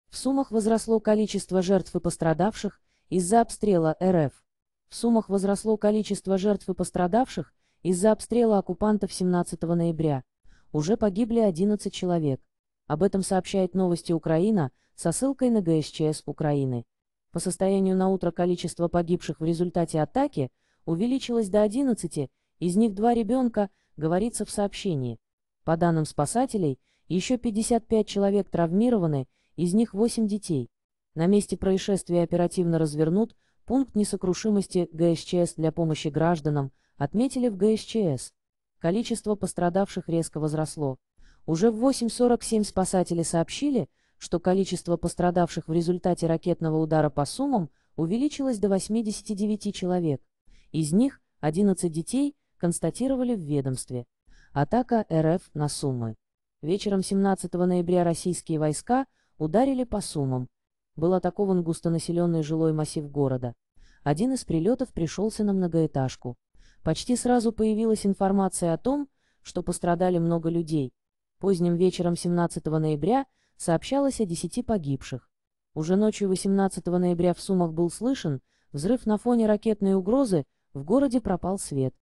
В Сумах возросло количество жертв и пострадавших из-за обстрела РФ.
Сумскаяобласть ВойнавУкраине В Сумах возросло количество жертв и пострадавших из-за обстрела оккупантов 17 ноября. Уже погибли 11 человек. Об этом сообщает Новости Украина со ссылкой на ГСЧС Украины. "По состоянию на утро количество погибших в результате атаки увеличилось до 11, из них 2 ребенка", - говорится в сообщении. По данным спасателей, еще 55 человек травмированы, из них 8 детей. "На месте происшествия оперативно развернут пункт несокрушимости ГСЧС для помощи гражданам", - отметили в ГСЧС. . Количество пострадавших резко